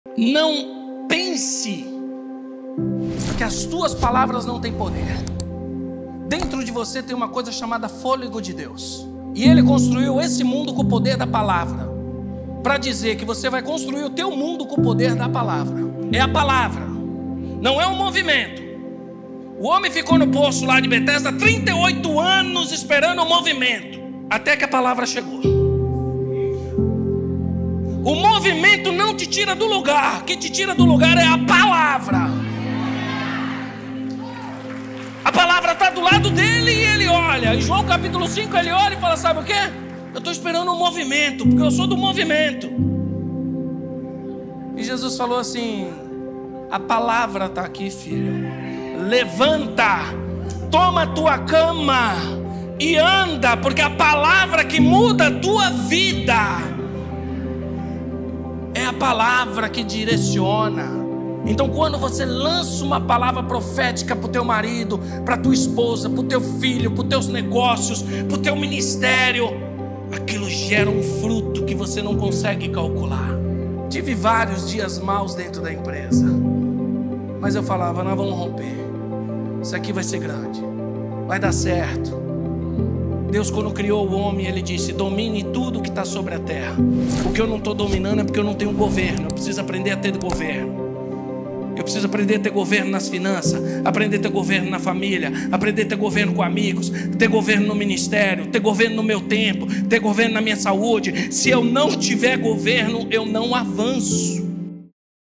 É isso que transforma a sua vida | Cesar Zanoni
Deus criou todo o universo através da palavra, e assim também será com você. Comece a declarar as bênçãos que espera da parte de Deus, pois é por meio das suas palavras, guiadas pela fé, que o seu mundo será transformado. O que você profetiza hoje moldará o seu amanhã!

Capítulos:

00:00 - Introdução
00:16 - Poder da palavra
01:31 - Conclusão

• Converse com a Assessoria Pr. Cesar Zanoni